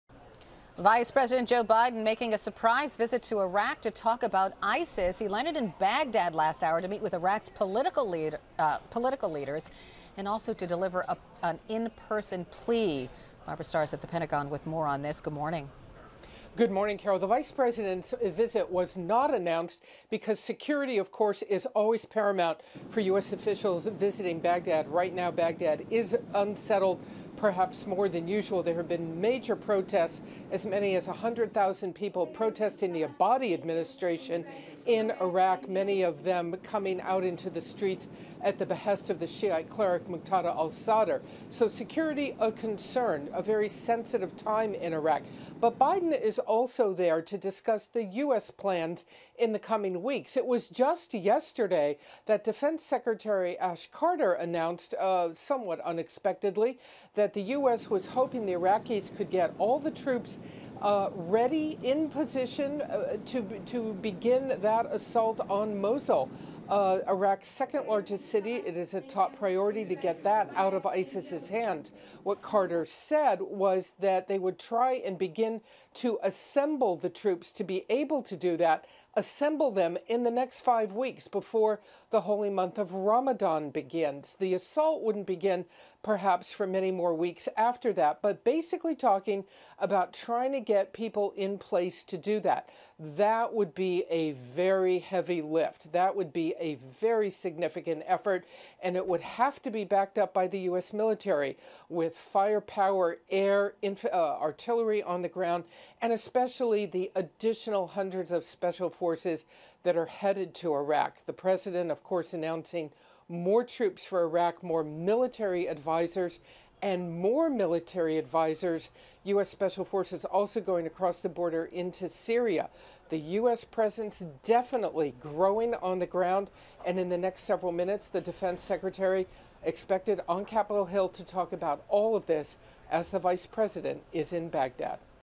Biden makes surprise Iraq visit
Vice President Joe Biden landed in Iraq Thursday for an unannounced trip where he will meet with political leaders.